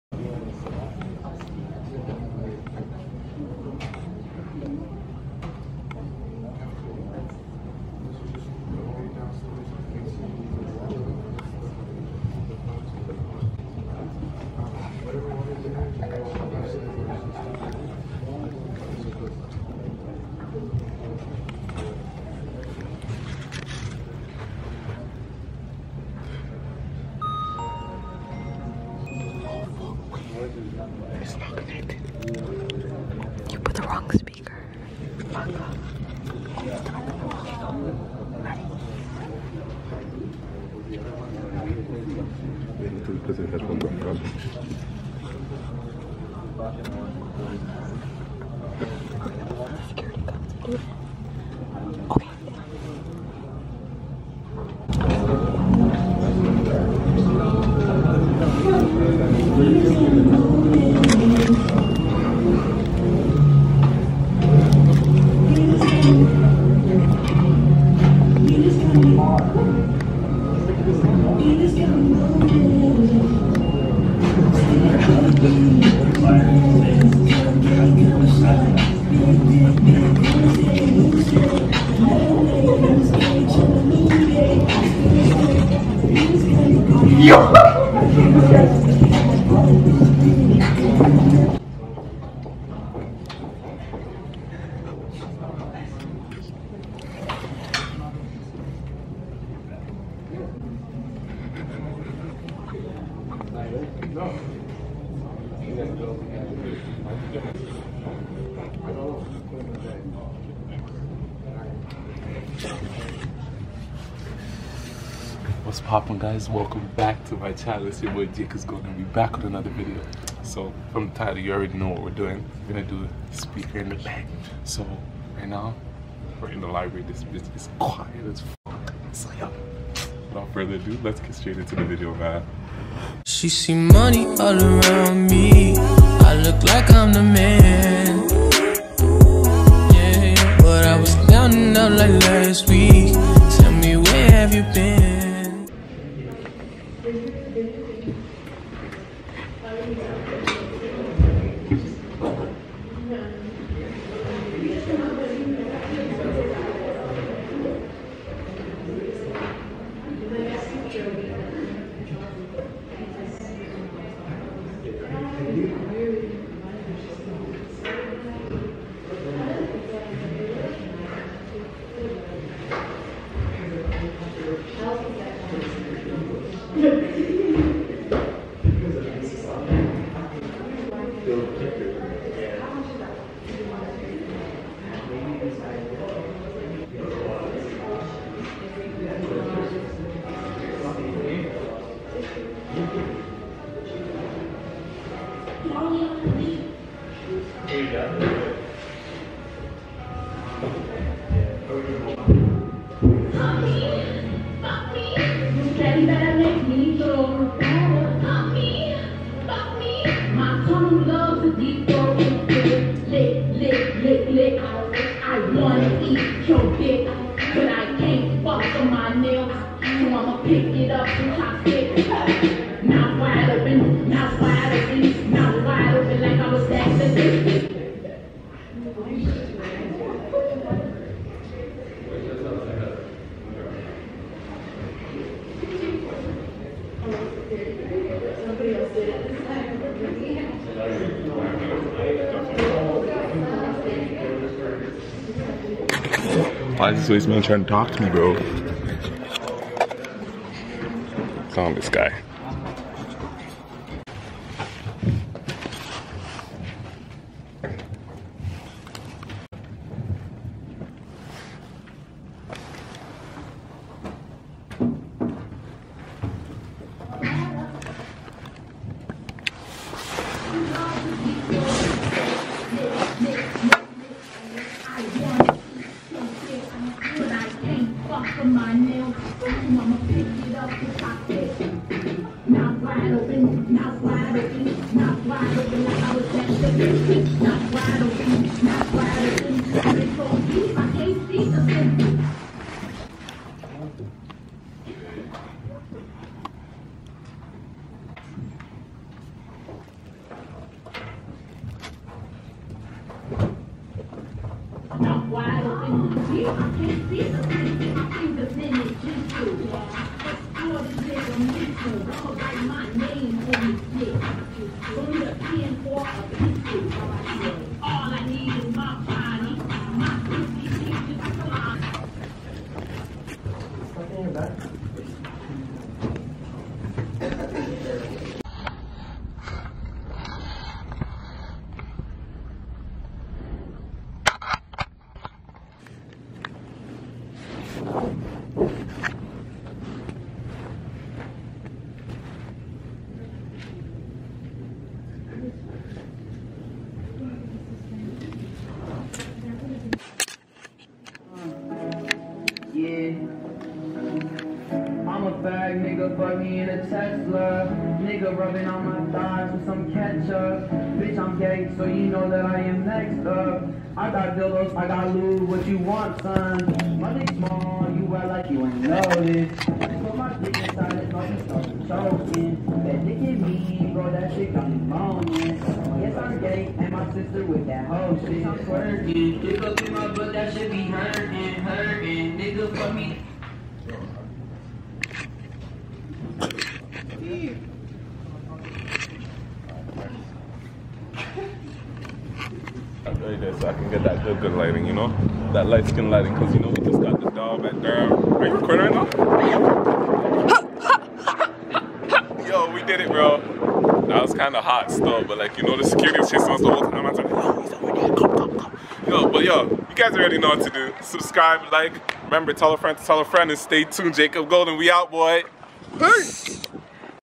Blasting Embarrassing Songs in Other People's Backpacks in the Library
Oh boy i really did it man!!!